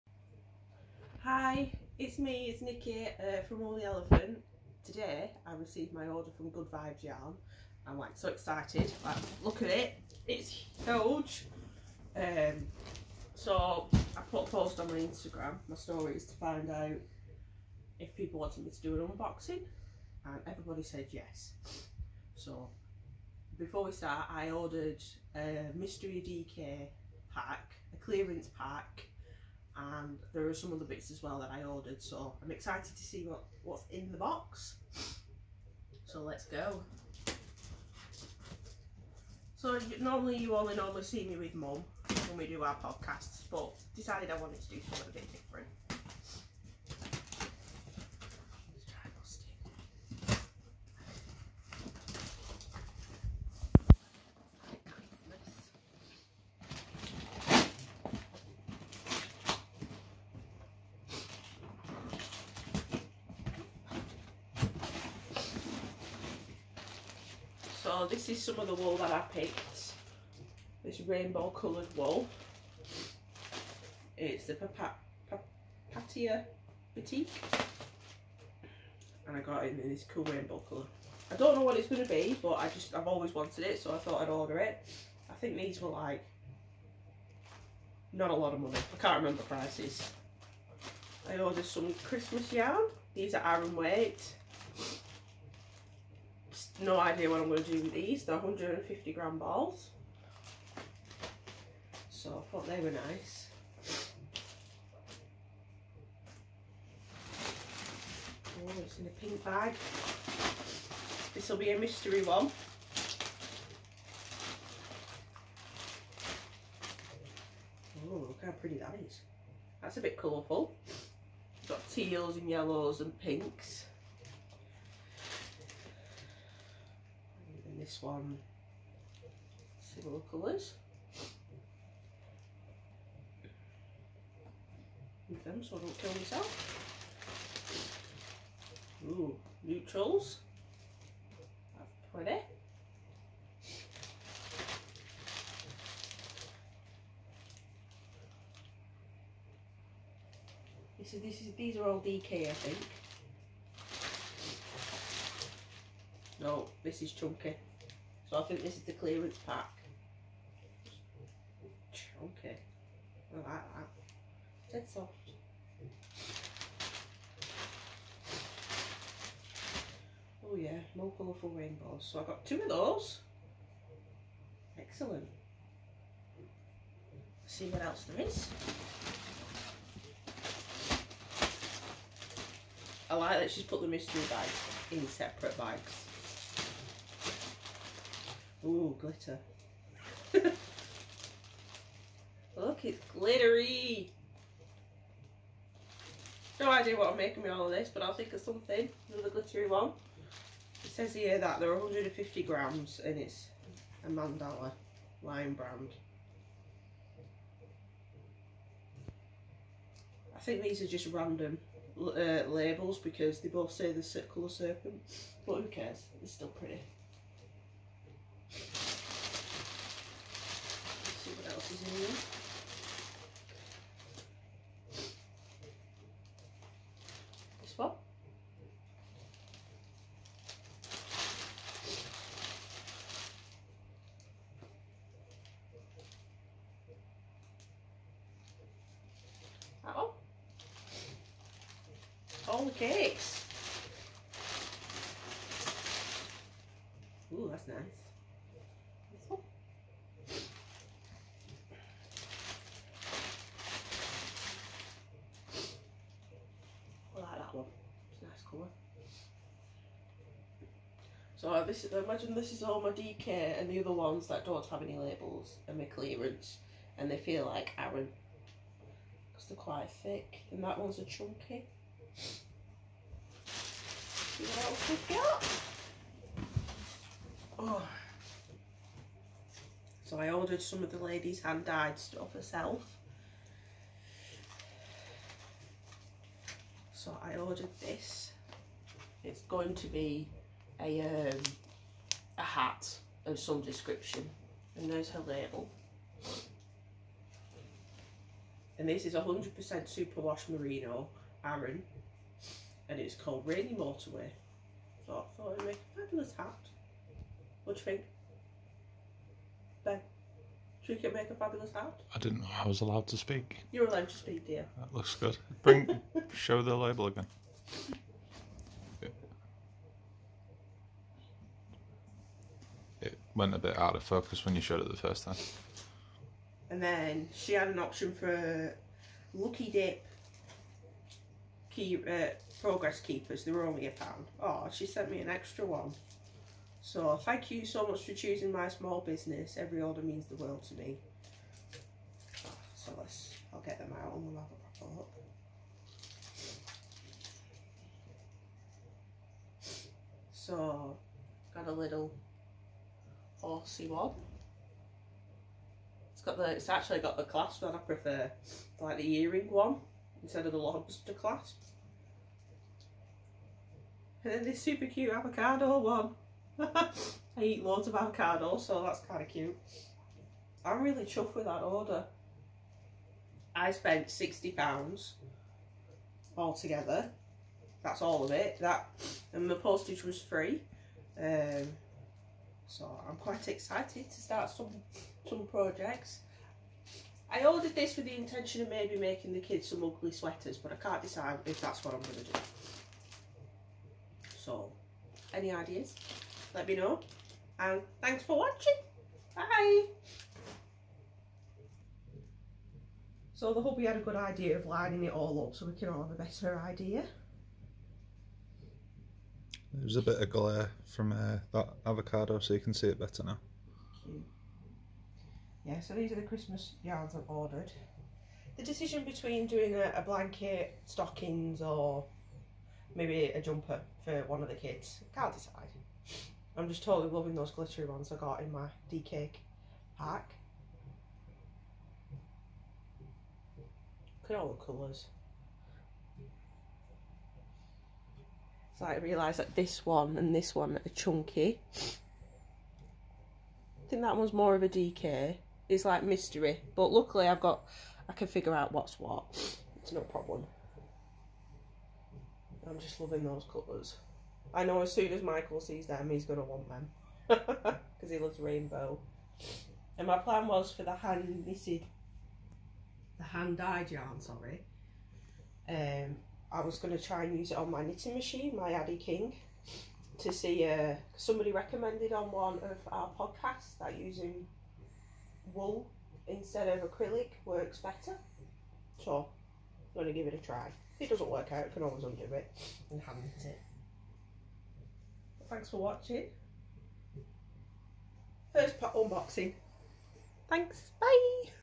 Unboxing my first Good Vibes yarn order. 😍
So here I am unboxing my first order from good vibes yarn. I bought all this with my own money, and wanted to share on video what I got.

I ordered a dk cake mystery pack, and clearance pack. I chose two aran cakes, 4 balls of DK and some hand dyed yarn too.